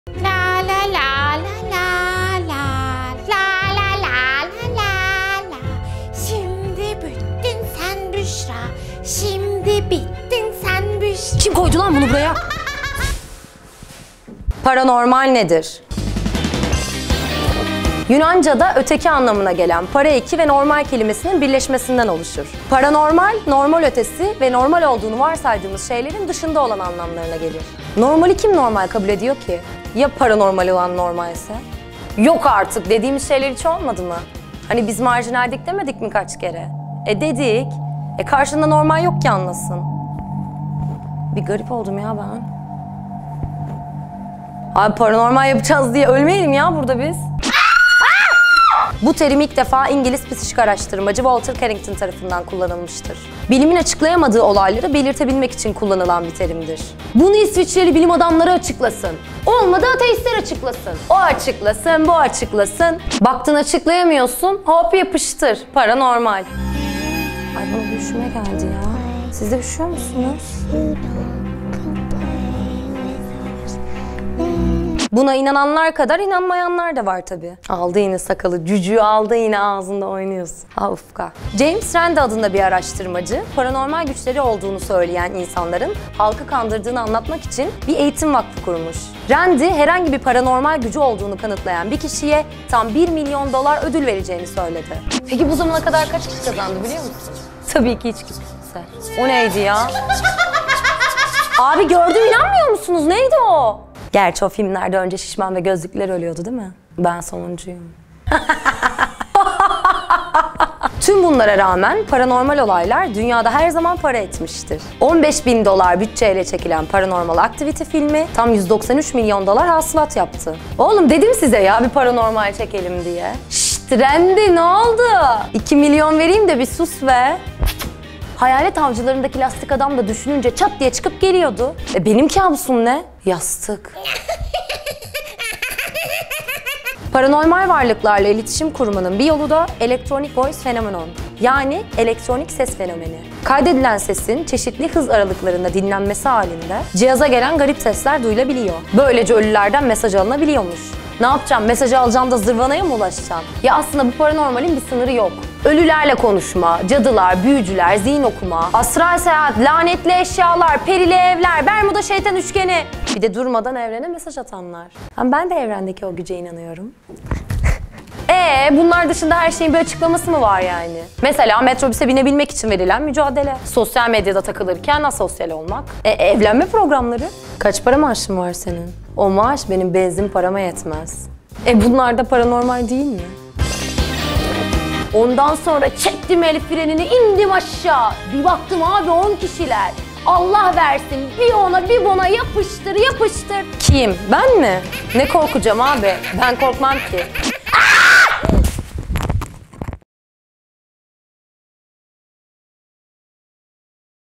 Paranormal Nedir?
Yunancada “ötesi” anlamına gelen para eki ve normal kelimesinin birleşmesinden oluşur. Paranormal, normal ötesi, olağan dışı, normal olduğunu varsaydığımız şeylerin dışında olan anlamlarına gelir. Normali kim normal kabul ediyor ki? Ya paranormal olan normalse. “yok artık” dediğimiz şeyler olmadı mı? Hani biz marjinaldik demedik mi kaç kere? Dedik. E karşında normal yok ki anlasın.

Bu terim ilk olarak ingiliz psişik araştırmacı walter whately carington tarafından kullanılmıştır. Bilimin açıklayamadığı olayları belirtebilmek için kullanılan bir terimdir. İsviçreli bilim adamları bunu açıklasın!
Buna inanan kadar inanmayanlar da var tabii. James randi adında bir araştırmacı, paranormal güçleri olduğunu söyleyen insanların halkı kandırdığını anlatmak için bir eğitim vakfı kurdu. Randi herhangi bir paranormal veya doğaüstü bir güce sahip olduğunu kanıtlayan kişiye, tam 1 milyon dolar ödül vereceğini söyledi. Peki, bugüne kadar ödülü kaç kişi kazandı? Tabii ki hiç kimse...
Tüm bunlara rağmen paranormal olaylar dünyada her zaman para etmiştir. Nasıl mı? 15.000 dolar bütçeyle çekilen paranormal activity filmi 193 milyon dolar hâsılat yapmıştır.
Kaydedilen sesin çeşitli hız aralıklarında dinlenmesi halinde cihaza gelen garip sesler duyulabiliyor. Böylece ölülerden mesajlar alınabiliyormuş.
 Ya aslında bu paranormalin sınırı yok. Ölülerle konuşma, cadılar, büyücüler, zihin okuma, durugörü, telekinezi, telepati, astral seyahat, lanetli eşyalar, bermuda şeytan üçgeni, perili evler, bi de durmadan evrene mesaj atanlar…

Nedir Snapchat: media.321

Yapım: 321Media
Sunan: Büşra Şenol
Yazan: Aida Ruhani